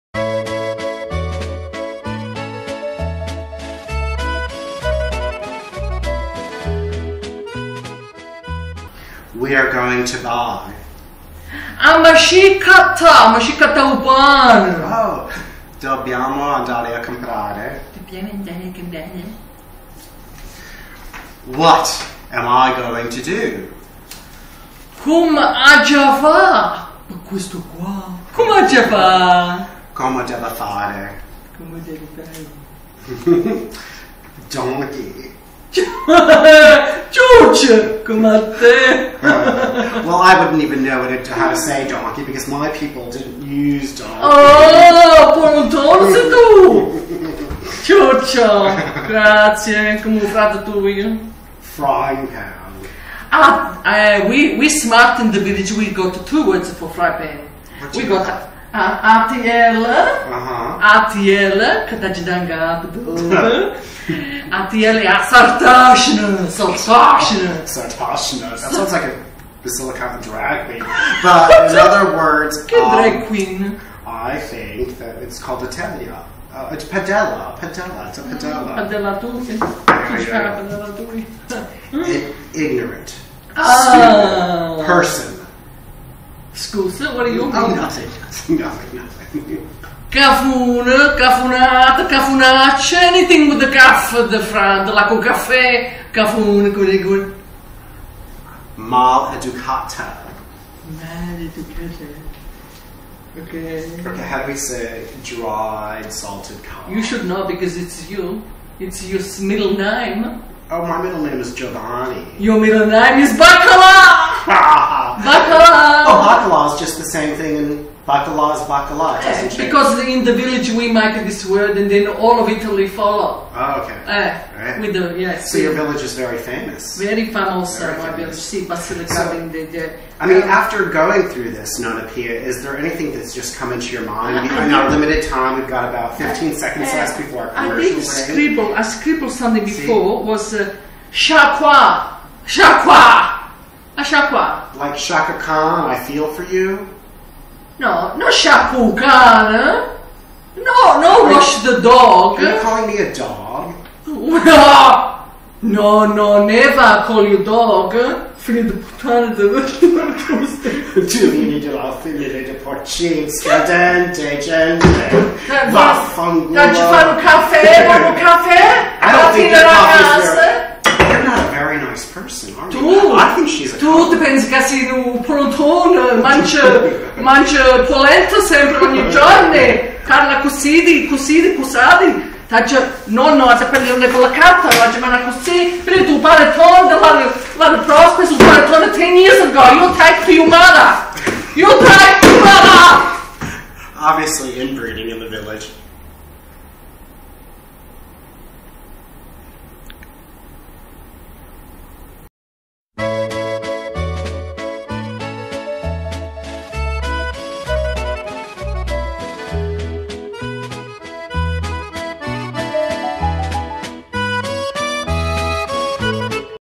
Nonna Pina - Teaches Dialect Part 2
The Showdown! Part 2 of the interview by Stu Cazzo (Stuart Katz) Nonna Teaches him words and phrases from her dialect.